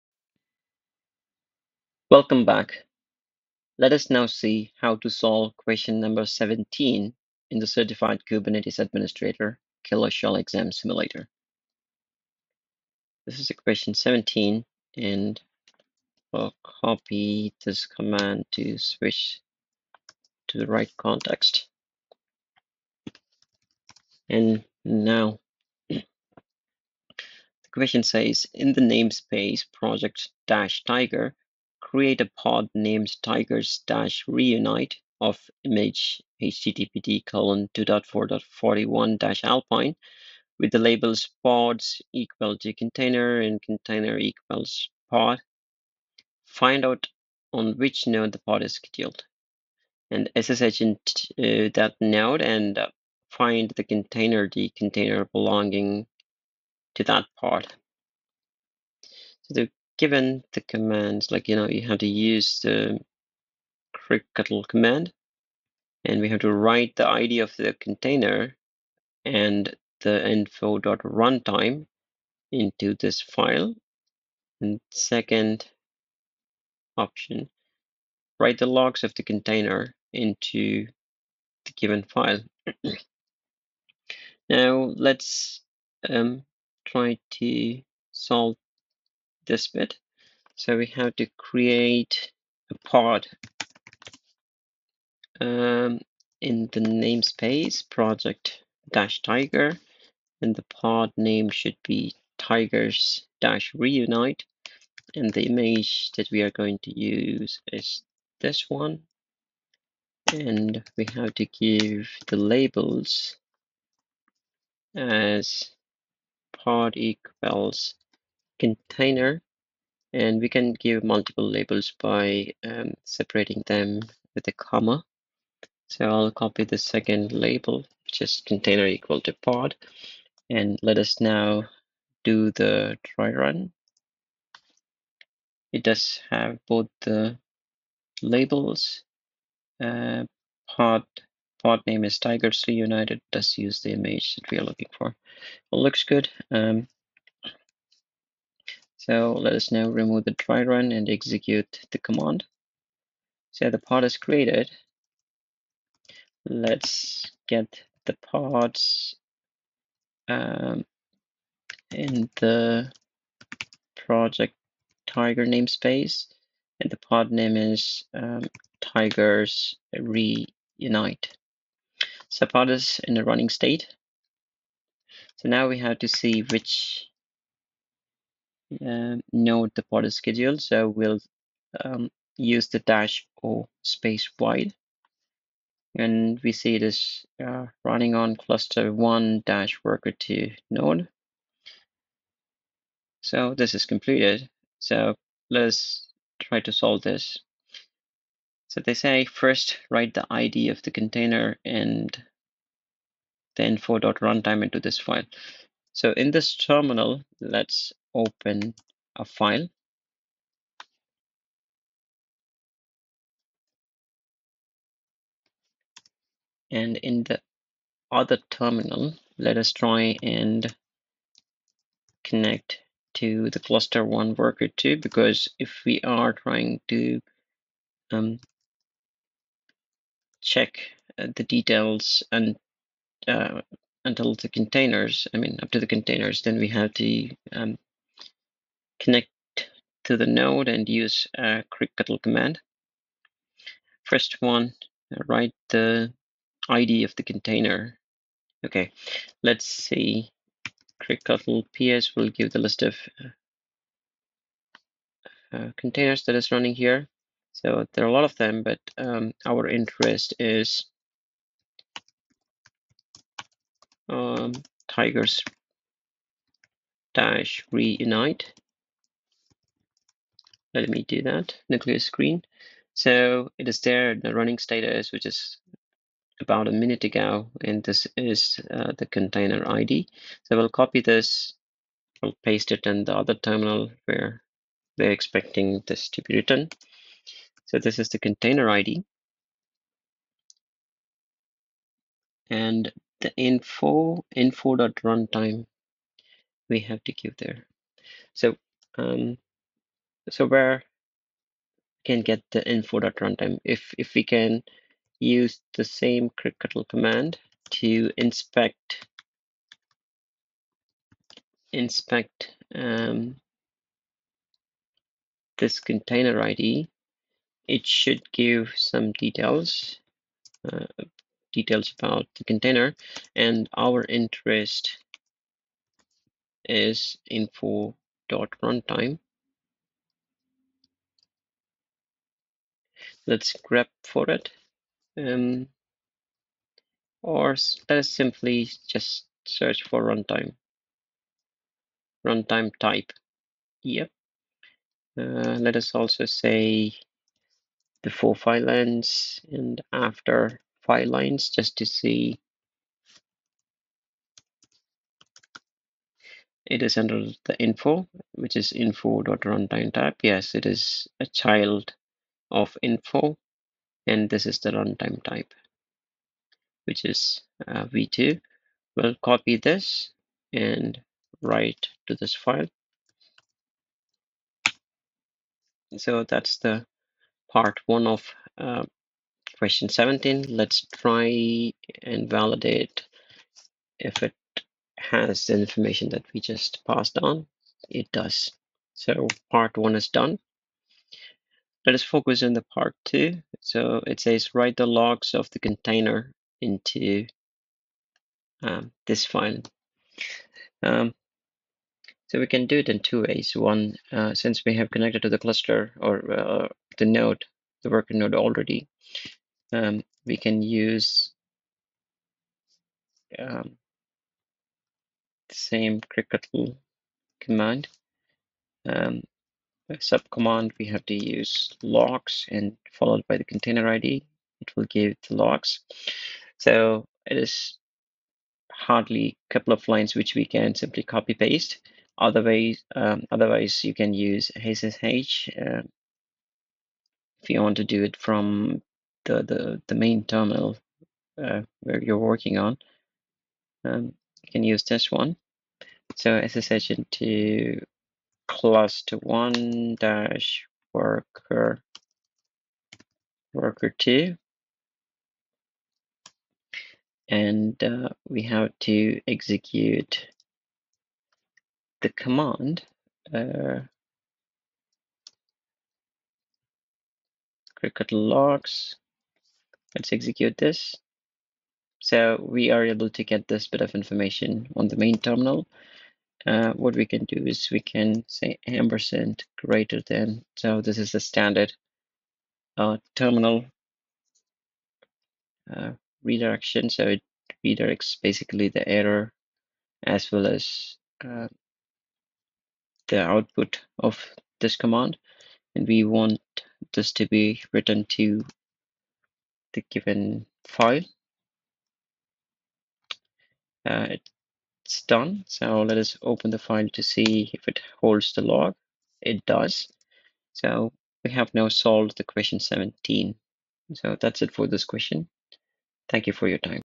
17 - CKA Question 17 Killer Shell
Killer.sh (Killer shell) - CKA Question 17
This video shows how to approach and solve the Question 17 in the Killer Shell CKA (Certified Kubernetes Administrator) exam simulation.